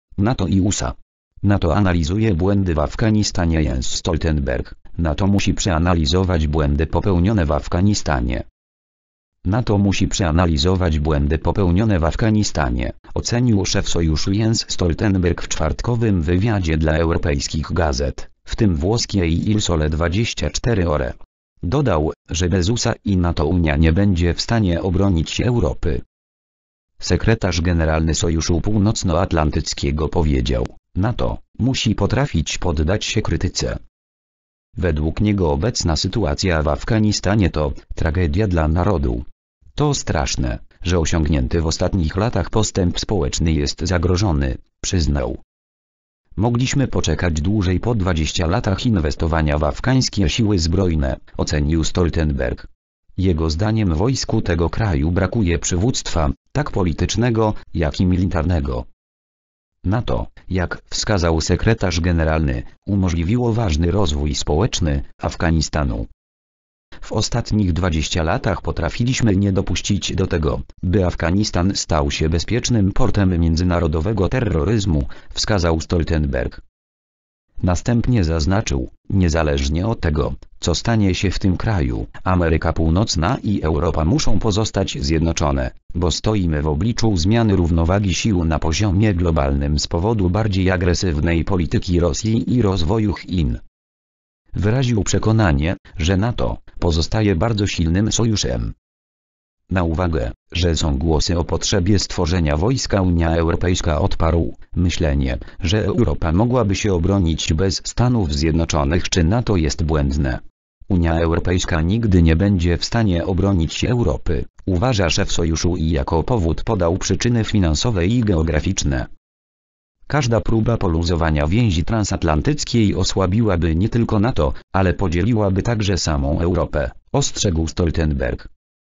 NATO i USA. NATO analizuje błędy w Afganistanie.
Jens Stoltenberg: NATO musi przeanalizować błędy popełnione w Afganistanie.

  (fot. PAP/EPA/FRANCISCO SECO)